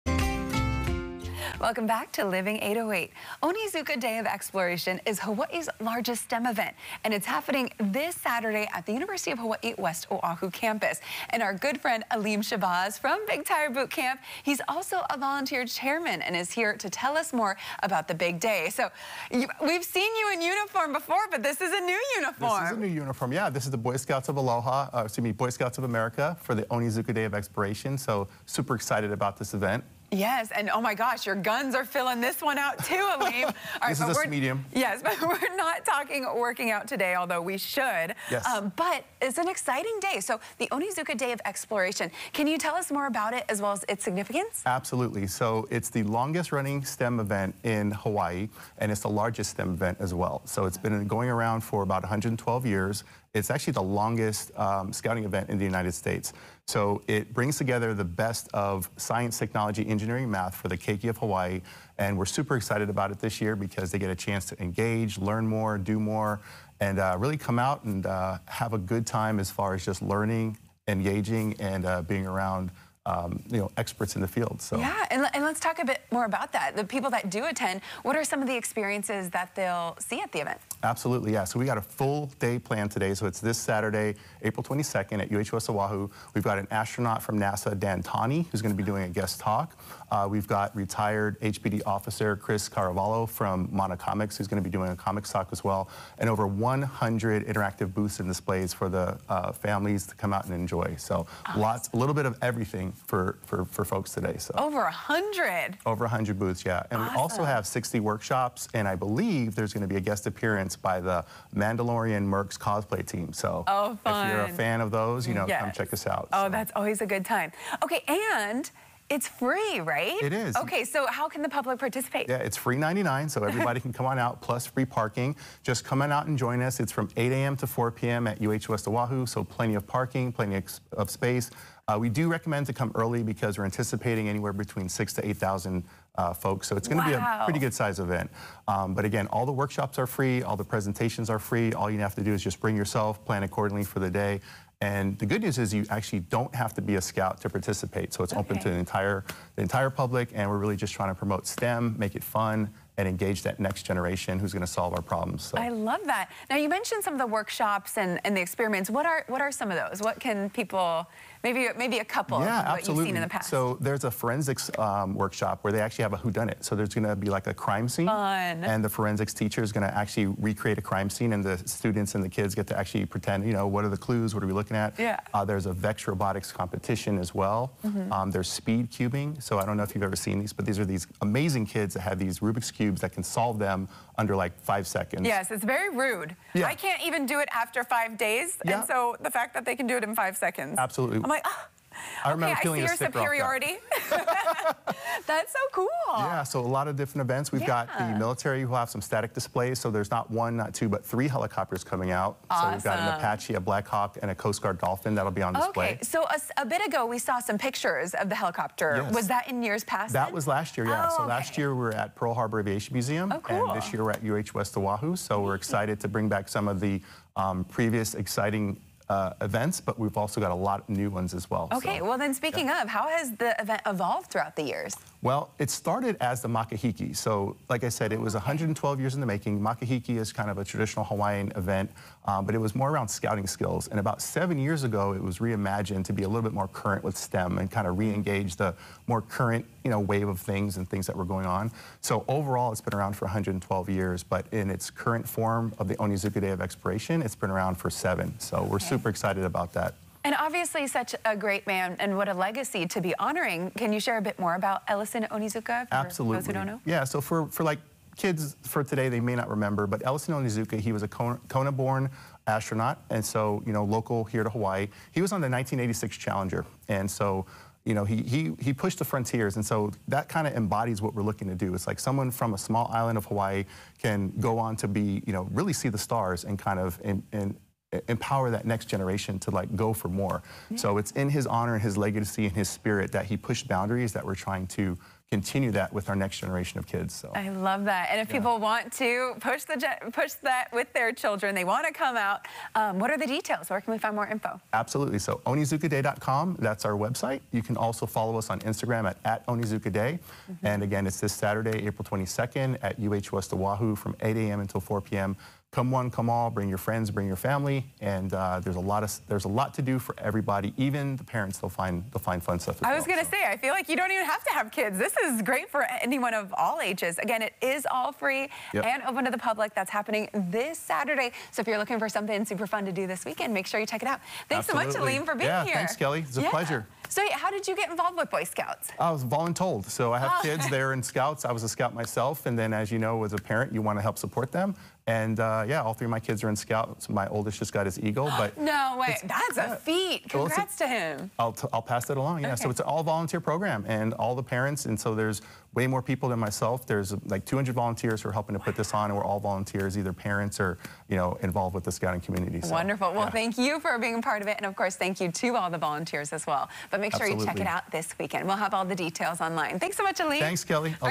Celebrate Onizuka Day of Exploration this weekend
The Onizuka Day of Exploration is an annual event honoring local astronaut Ellison Onizuka, an Eagle Scout raised in Kona and was on the Challenger space mission. Alim Shabazz, volunteer Chairman of Onizuka Day joined us to talk about why this event is so important.